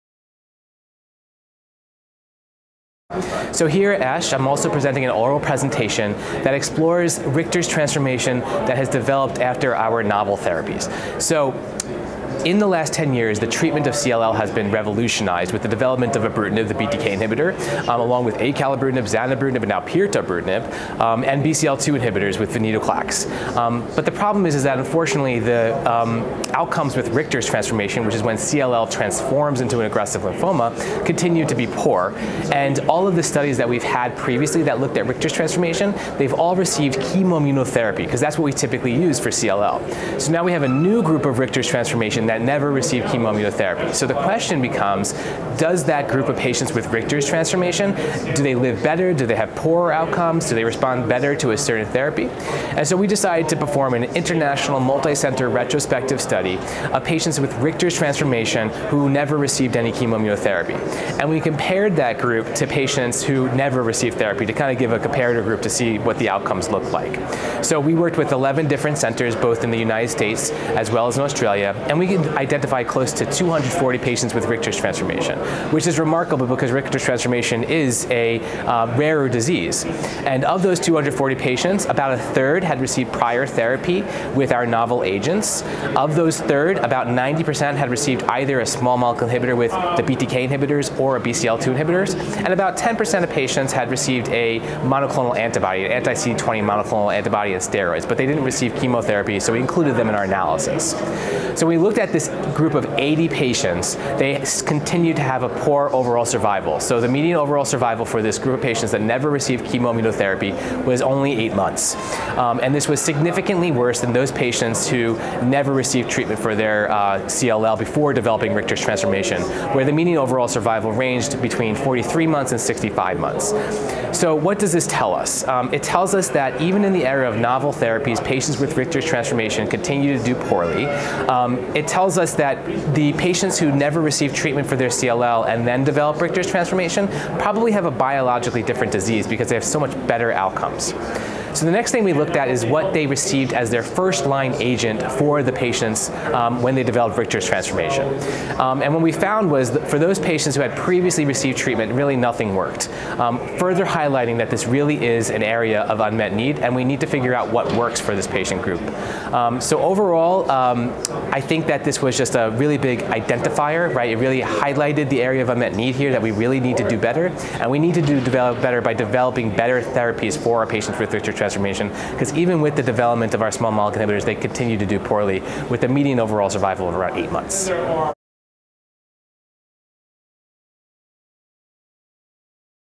Outcomes of patients with Richter's transformation without prior chemoimmunotherapy for CLL/SLL
Adam Kittai, MD, Ohio State University, Columbus, OH, comments on a multicenter retrospective study which investigated the variables predictive of survival in Richter’s transformation patients who did not undergo prior chemoimmunotherapy (CIT) for their chronic lymphocytic leukemia/small lymphocytic lymphoma (CLL/SLL) and have been treated exclusively with small molecule inhibitors (SMI) (BTK and BCL2 inhibitors). Those patients who did not undergo any treatment for CLL/SLL prior to RT (both CIT and/or SMI), had greater disease outcomes than those who were treated for their CLL/SLL, highlighting the need to improve treatment of patients who are not treatment naïve at the time of transformation. This interview took place at the 65th ASH Annual Meeting and Exposition, held in San Diego, CA.